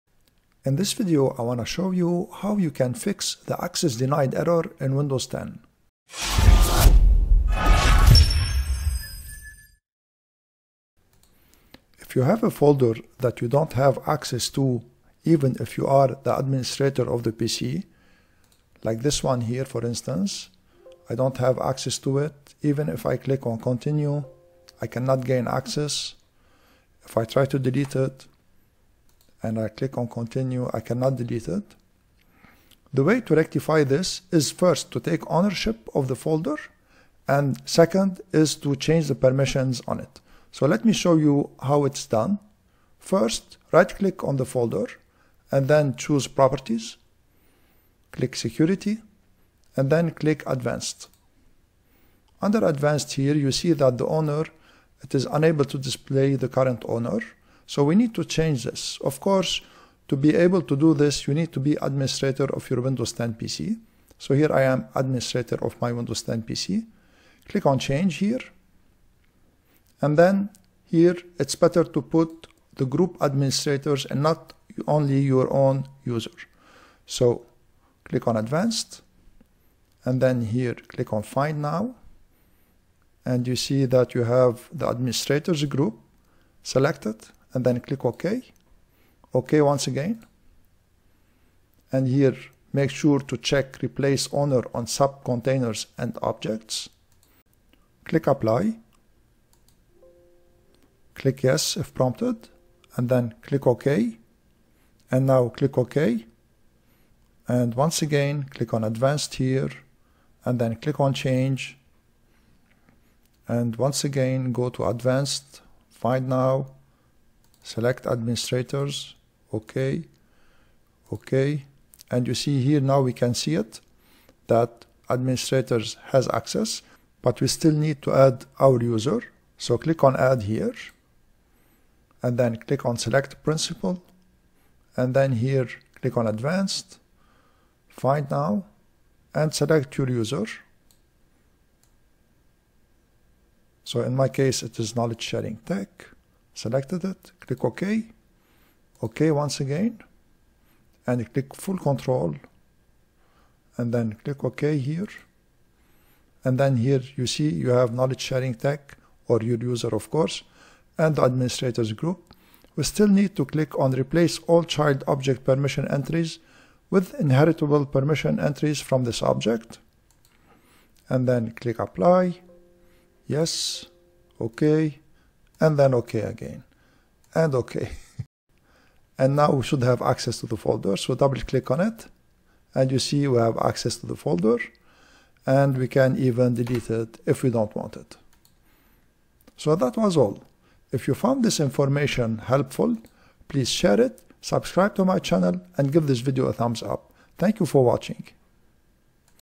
Fix Access Denied error in Windows 10 // Easy step by step guide
Fix permission error in Windows 10 "You don't currently have access to this folder". Take ownership of files and change their permission in Windows 10 by following the instructions in my video. Gain access to files in Windows 10 by changing their permission and owner.

Important Notes:
- Use the instructions in this video on your own files and do not use it for malicious purpose.
- Do not change the permissions or the ownership on system files unless you know what you are doing

Follow me on:
My websites: